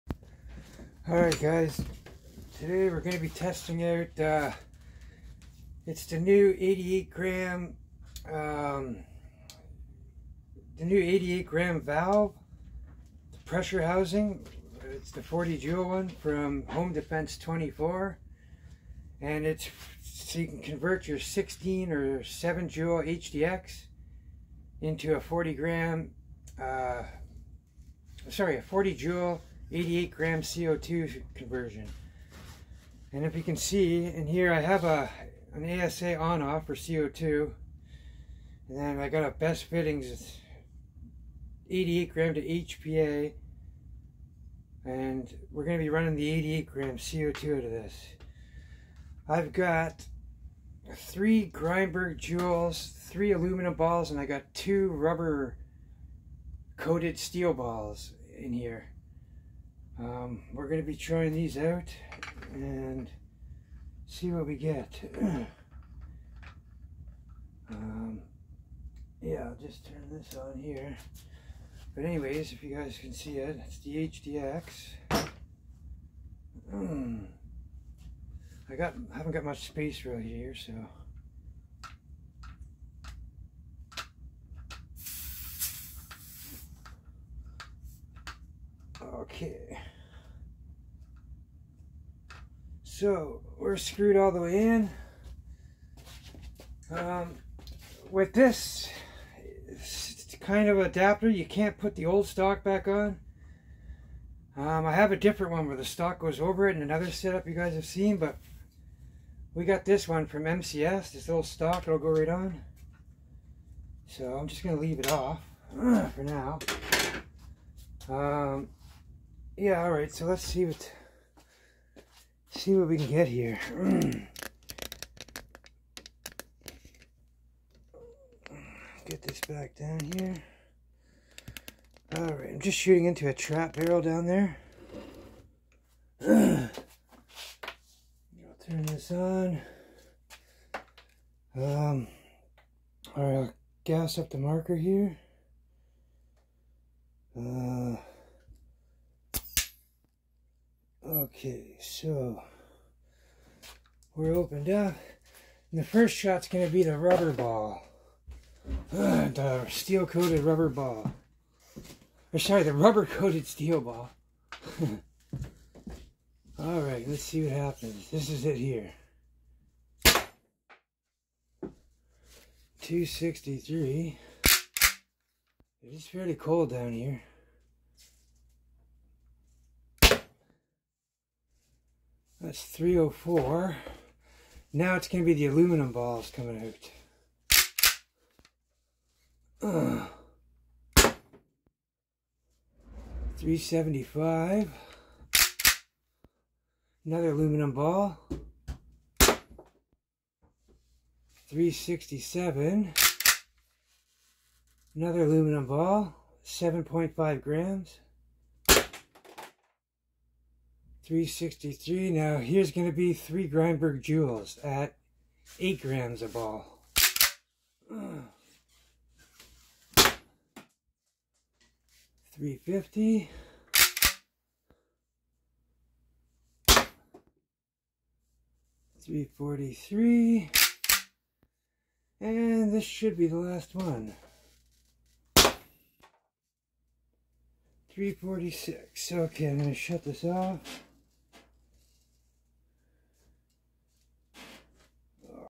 Umarex HDX 68 Drop in 88g Valve Power Test Home Defence 24 49 Joules
this video got cut short so if you have any questions just ask, today i test the hdx 68 new drop in valve 40 joule version, my hdx was a 7 joule version before i added this valve, we test 3 different ammos and got 49 joules with aluminum balls and grimburg juls and steel coated rubber balls, also you can use the stock that comes with the hdx as long as you dont use the asa on off, the stock still fits perfectly, you can get the new drop in valve and ammo